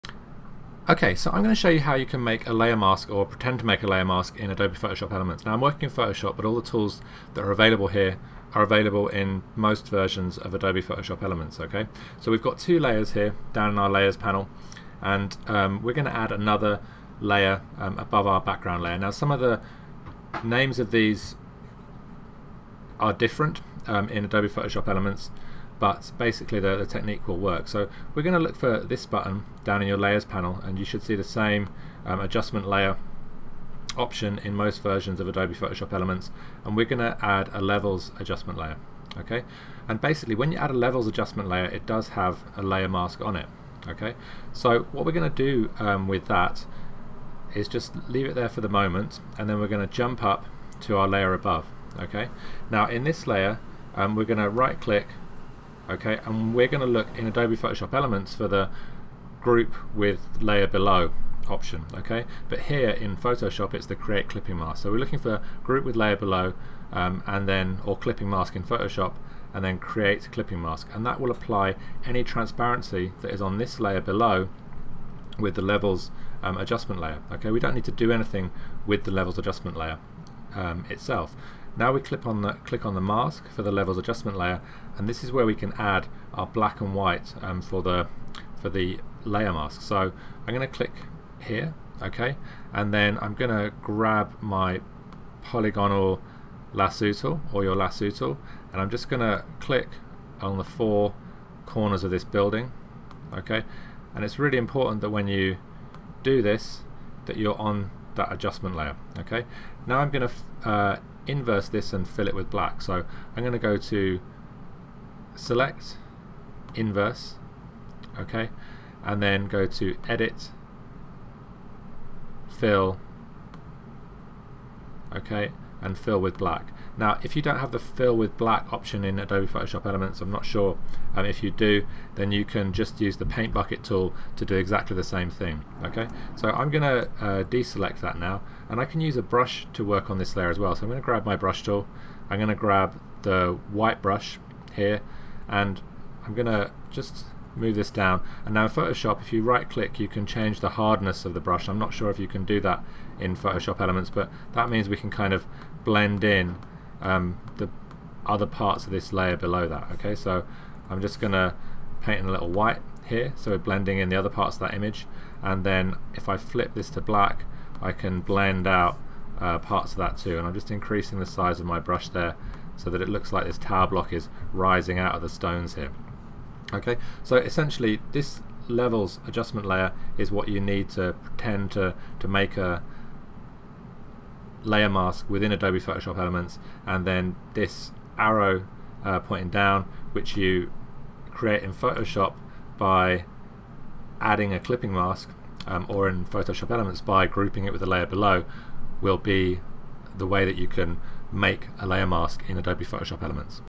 Photoshop Elements: Create a 'Layer Mask' in Photoshop Elements
This demonstration for older versions of Photoshop Elements (done in Adobe Photoshop) shows which tools you will need to make a 'Layer Mask' in Adobe Photoshop Elements. The tools that I run through are all available in most versions of Photoshop Elements.